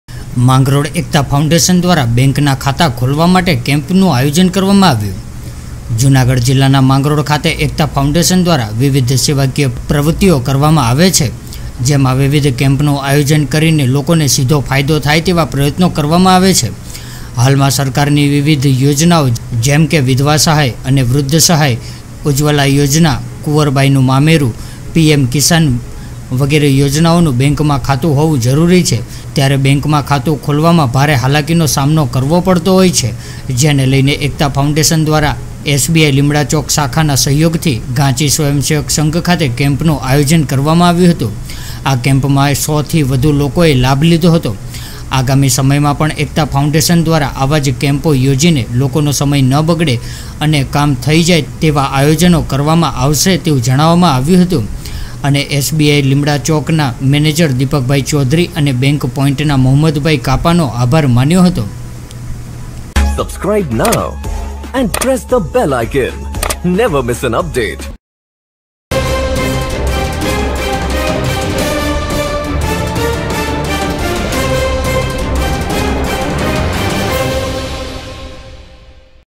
Mangrol News | માંગરોળ: એકતા ફાઉન્ડેશન દ્વારા Bank Account ખોલવા માટેનો કેમ્પ યોજાયો| Dpnews
MAngrol News | માંગરોળ એકતા ફાઉન્ડેશન દ્વારા બેંકના ખાતા ખોલવા માટેનો કેમ્પ યોજાયો| Dpnews

About Video: માંગરોળ એકતા ફાઉન્ડેશન દ્વારા બેંકના ખાતા ખોલવા માટેનો કેમ્પ યોજાયો

જૂનાગઢ જિલ્લાના માંગરોળ ખાતે એકતા ફાઉન્ડેશન દ્વારા વિવિધ સેવાકીય પ્રવૃતિઓ કરવામાં આવે છે, વિવિધ કેમ્પનું આયોજન કરી લોકોને સીધો ફાયદો થાય તેવા પ્રયત્ન કરવામાં આવે છે.
હાલમાં સરકારની વિવિધ યોજનાઓ જેમકે વિધવા અને વૃદ્ધ સહાય, ઉજવવલા યોજના, કુંવરબાઈનું મામેરું, પીએમ કિસાન વગેરે યોજનાઓમાં બેંકનું ખાતું હોવું જરૂરી છે ત્યારે બેંકમાં ખાતું ખોલાવવામાં ભારે હાલાકી નો સામનો કરવો પડે છે જેને લઈ એકતા ફાઉન્ડેશન દ્વારા એસબીઆઈ લીમડાચોક શાખાના સહયોગ થી ઘાંચી સ્વંય સંઘ ખાતે કેમ્પ નું આયોજન કરવામા આવ્યું હતું..
આ કેમ્પમાં ૧૦૦ થી વધુ લોકોએ લાભ લીધો હતો.
આગામી સમયમાં પણ એકતા ફાઉન્ડેશન દ્વારા આવા કેમ્પ યોજી લોકોનો સમય ન બગડે અને કામ થઈ જાય તેવા આયોજનો કરવામા આવશે તેવું જણાવવામાં આવ્યુ હતું અને એસબીઆઈ લીંમડાચોકના મેનેજર દિપક ચૌધરી અને બેંક પોઇન્ટના મોહમદ ભાઈ કાપાનો આભાર માન્યો હતો..